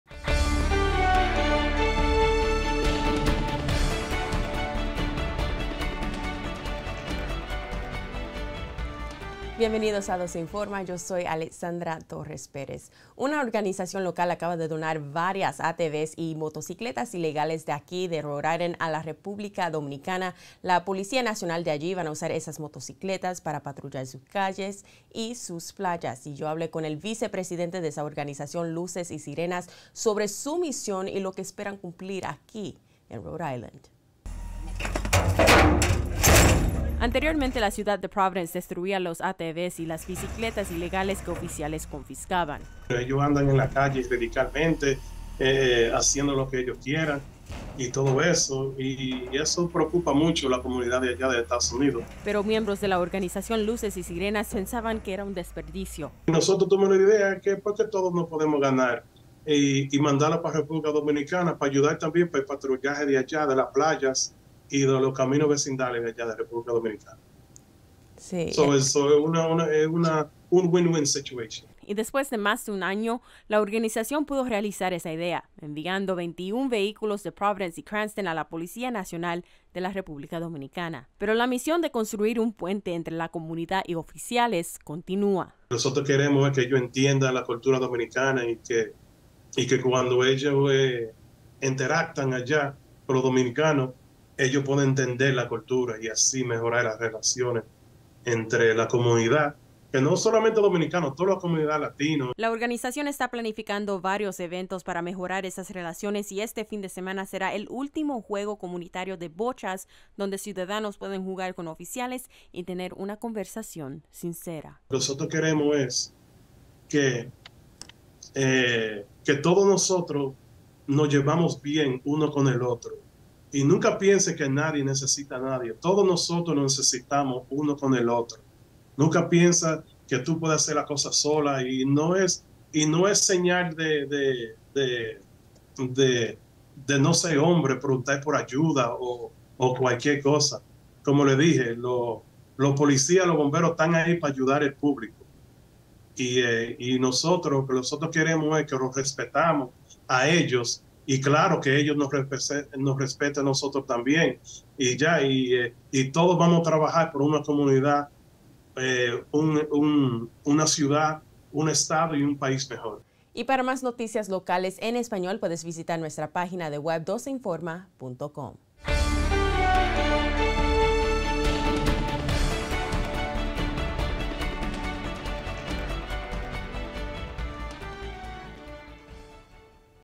ATVs y bicicletas ilegales donadas para policía nacional de Republica Dominicana
Una organización local envió  21 vehículos de Providence y Cranston a la Policía Nacional de la Republica Dominicana, pero la misión de construir un puente entre la comunidad y oficiales continua.

WPRI12's Spanish-language news and community program.